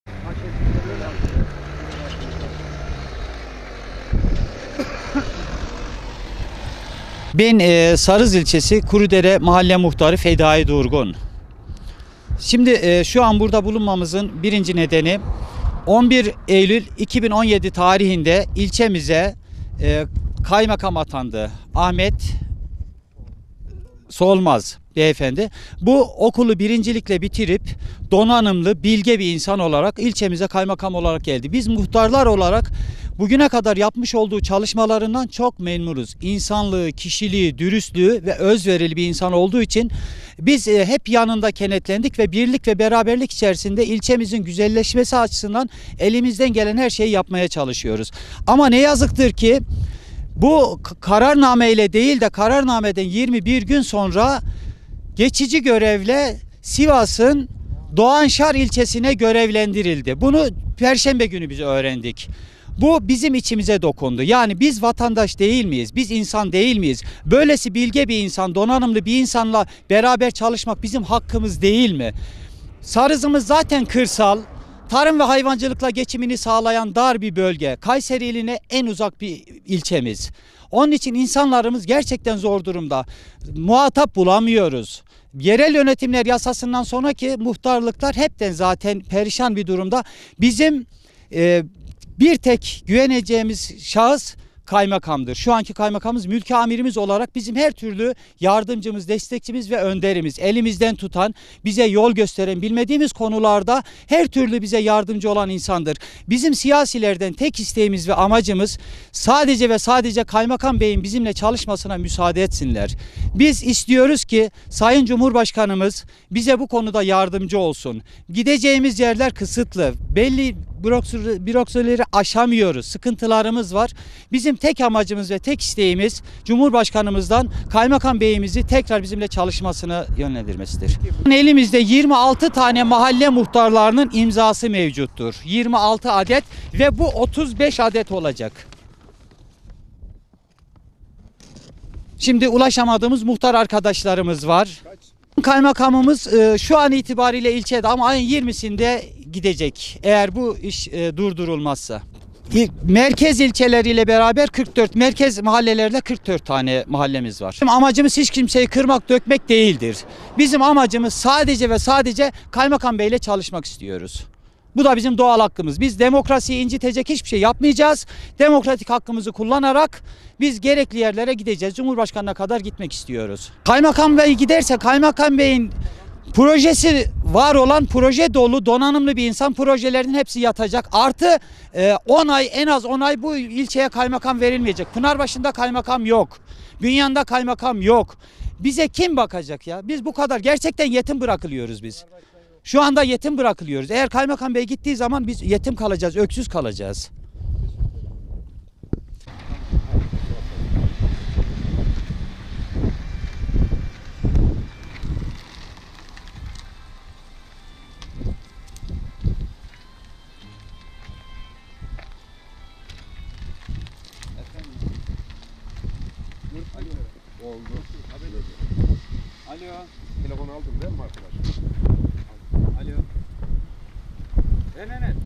Muhtarlar Kaymakamlarının ilçede kalması için imza topladı
Kayseri’nin, Sarız ilçesinde muhtarlar geçici olarak Sivas’ın Doğanşar ilçesine atanan Kaymakamları Ahmet Solmaz'ın, yeniden ilçelerinde görevlendirilmesi için imza topladı. Muhtarlar, hazırladıkları dilekçelerine mühürlerini de bastı.